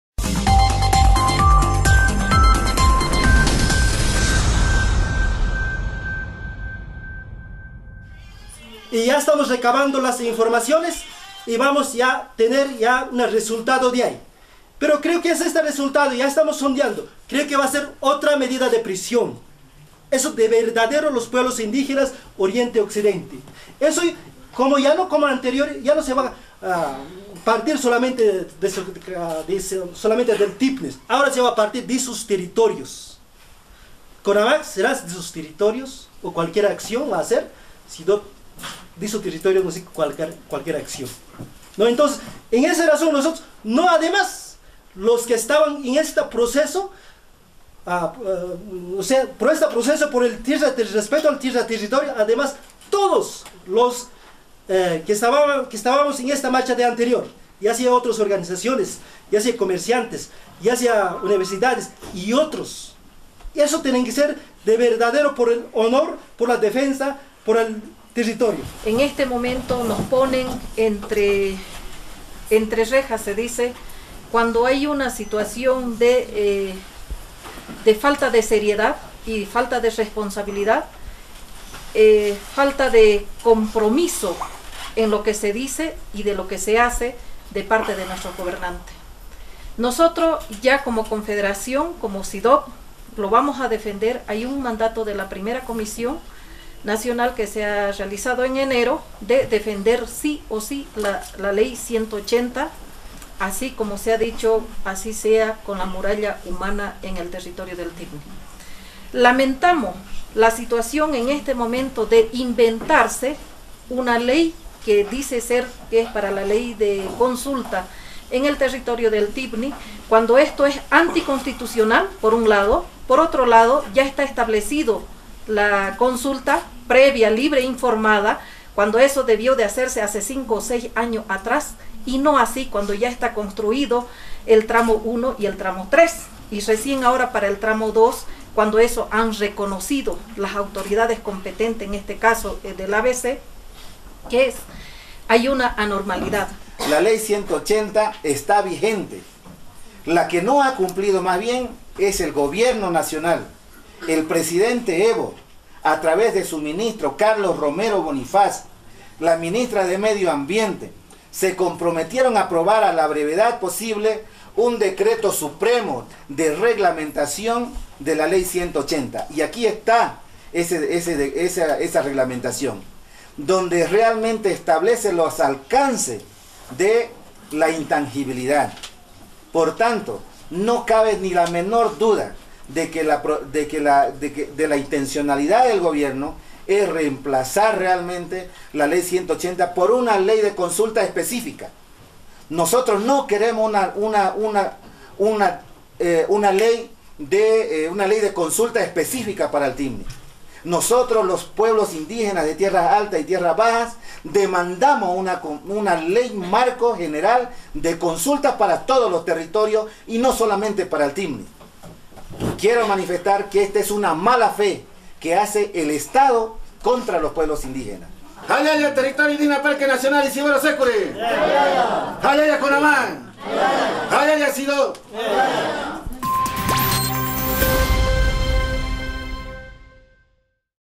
IX marcha Indígena
Indígenas en emergencia por el TIPNIS, rechazan consulta del MAS

La CIDOB y el CONAMAQ se mantienen en estado de emergencia por la posible anulación de la Ley Corta que protege el TIPNIS, con la propuesta de consulta que plantea el MAS. Anuncian la IX marcha desde sus comunidades y le recuerdan al gobierno que le demandaron la Ley Marco de Consulta y no una específica sobre el Parque.

ERBOL